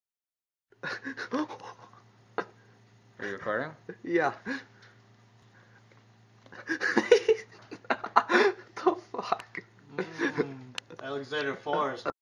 My Friend Eating The Biggest Moth Ever
wtf...i have dumb friends i swear...like for teh lulz?